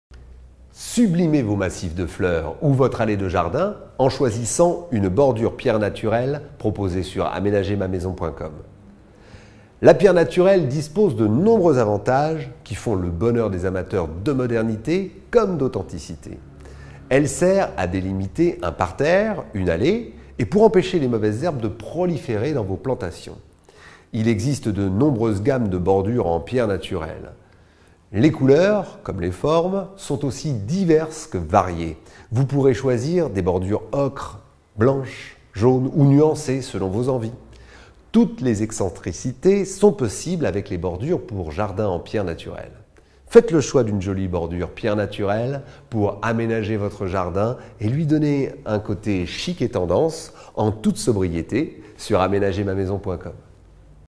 Bordure Pierre Naturelle Jardin et Terrasse
Sublimez vos massifs de fleurs ou votre allée de jardin en choisissant une bordure pierre naturelle proposée sur AMENAGER MA MAISON.COM !
La pierre naturelle dispose de nombreux avantages qui font le bonheur des amateurs de modernité comme d'authenticité.
Elle sert à délimiter un parterre, une allée et pour empêcher les mauvaises herbes de proliférer dans vos plantations.
Il existe de nombreuses gammes de bordures en pierre naturelle.
Les couleurs comme les formes sont aussi diverses que variées.
Vous pourrez choisir des bordures ocres, blanches, jaunes ou nuancées selon vos envies.
Toutes les excentricités sont possibles avec les bordures pour jardin en pierre naturelle.
Faites le choix d'une jolie bordure pierre naturelle pour aménager votre jardin et lui donner un côté chic et tendance en toute sobriété sur AMENAGER MA MAISON.COM !
Modifier